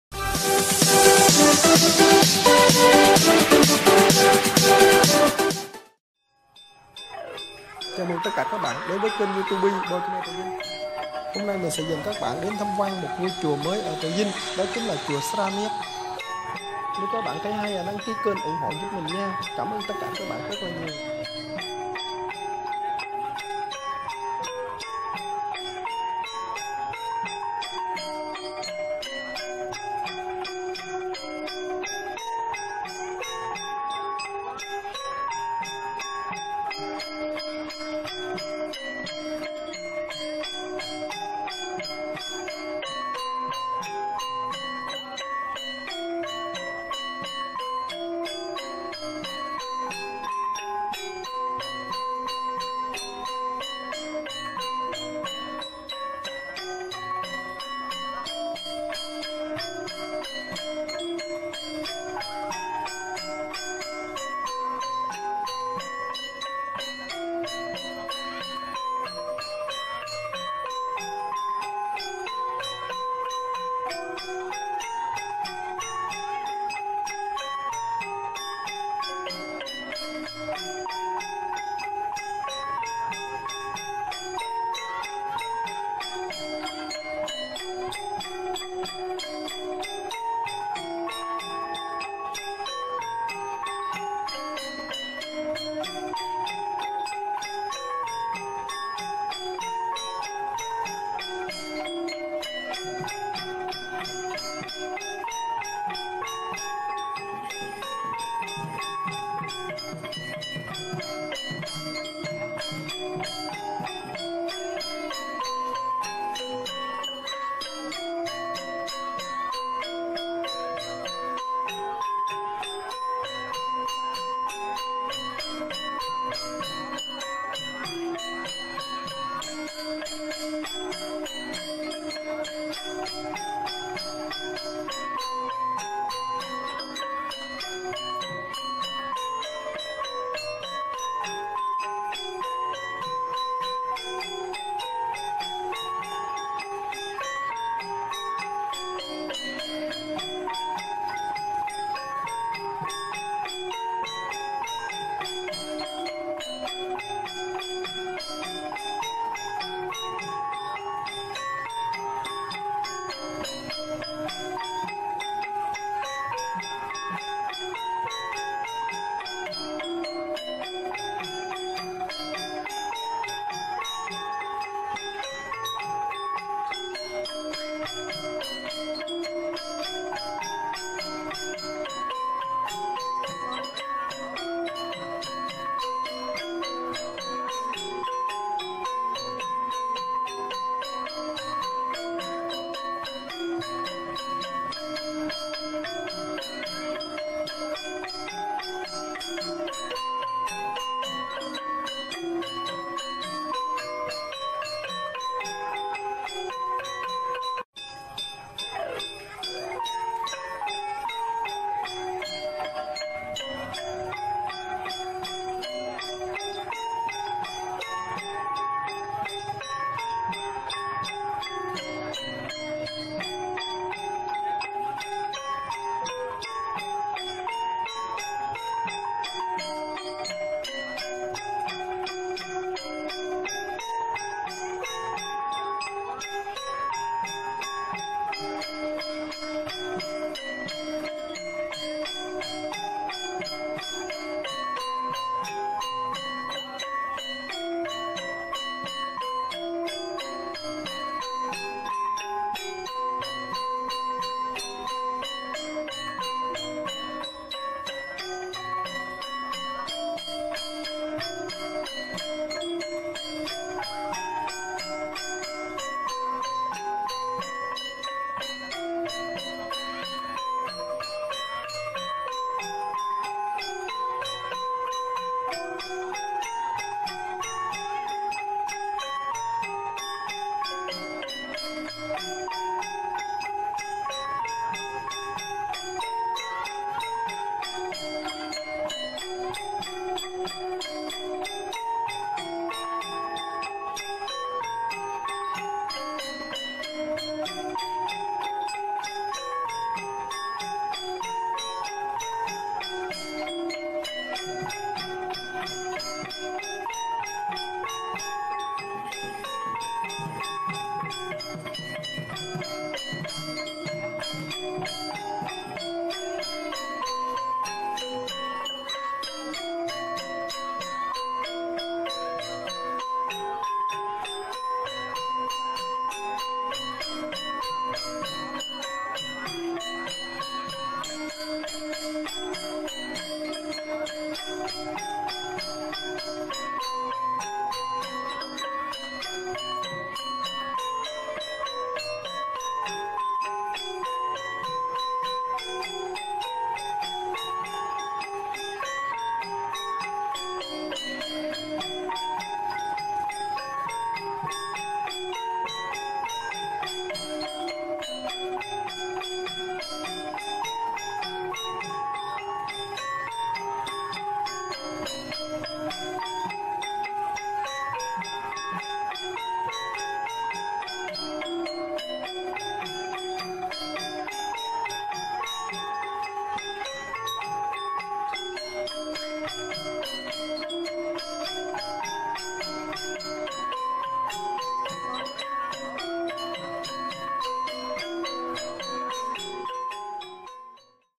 Chùa Sra Nek tại Trà Vinh | Boy Khmer Trà Vinh
Chùa Sra Net là một ngôi chùa khmer tại Trà Vinh, ngôi chùa được xây dựng rất đẹp.